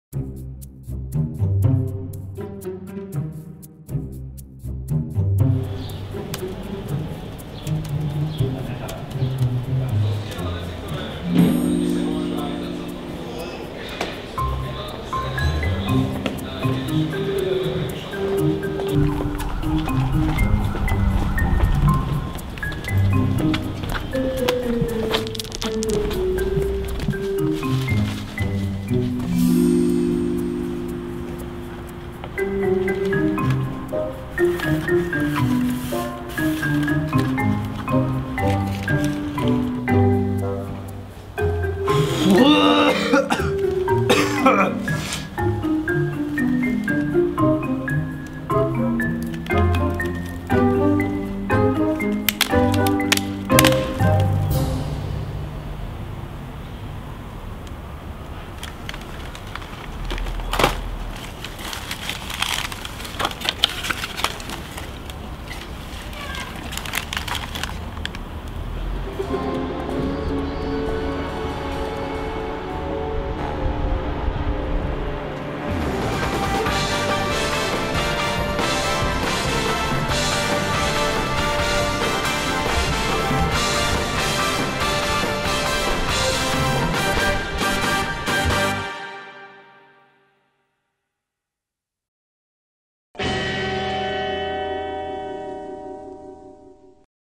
BEZDOMOVEC MÁ NOTEBOOK??
Síce krátke ... ale sme sa riadne pobavili :D :D :D Toto video je ak ťa nebaví info ale zábava. Skeč na testovaný notebook-Asus 360 Flip

ASK: ask.fm/TheBacaa